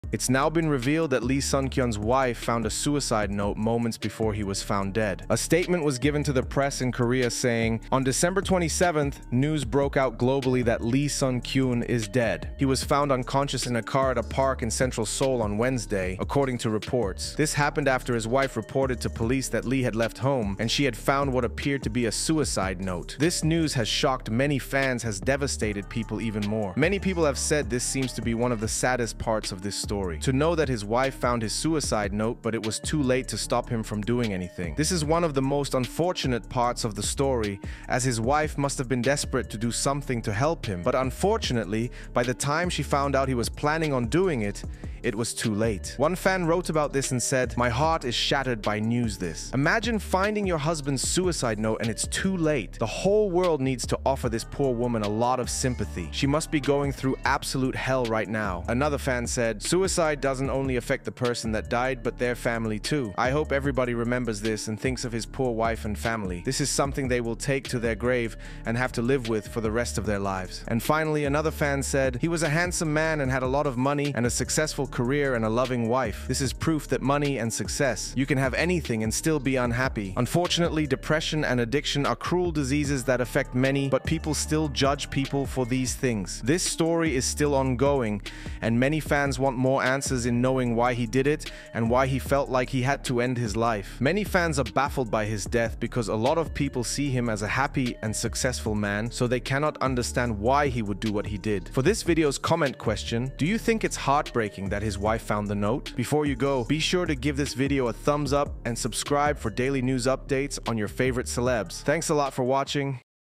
Lee Sun-kyun’s Wife Found Suicide Note Moments Before He Was Found Dead
Jeon Hye Jin had some very awful news today.